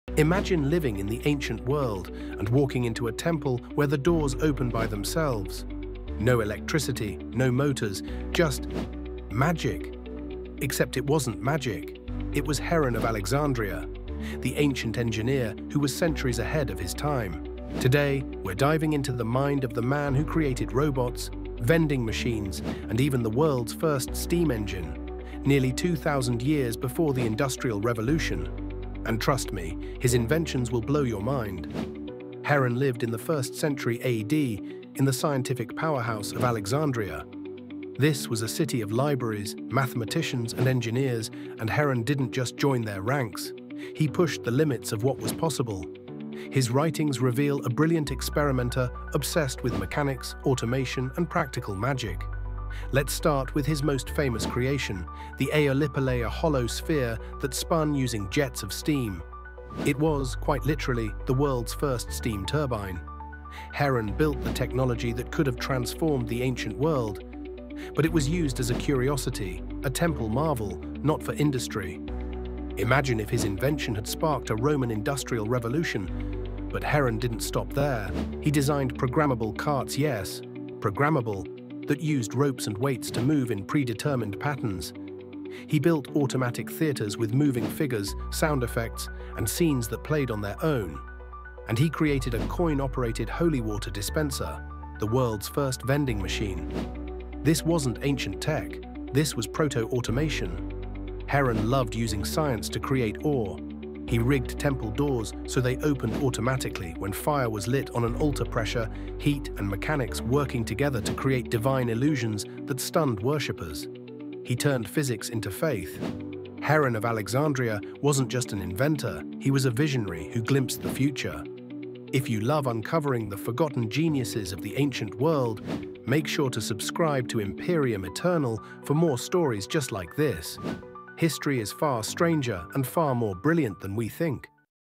Heron of Alexandria: The Ancient Inventor Who Built the First Robot & Steam Engine
"Heron of Alexandria was one of the greatest engineering minds of the ancient world—an inventor who created steam engines, robots, vending machines, and automated temples nearly 2,000 years before modern technology.

In this video, we explore Heron’s incredible machines, including the Aeolipile (the world’s first steam turbine), his programmable carts, ancient robotics, coin-operated devices, and the scientific illusions that amazed worshippers in Alexandria. f you love forgotten geniuses, ancient engineering, and Bronze Age mysteries, subscribe to Imperium Eternal for more history brought back to life.
Topics Covered:
Who was Heron of Alexandria?
The Aeolipile and early steam technology
Ancient robots and automatic theaters
Coin-operated inventions
Temple door automation
Why Heron was centuries ahead of his time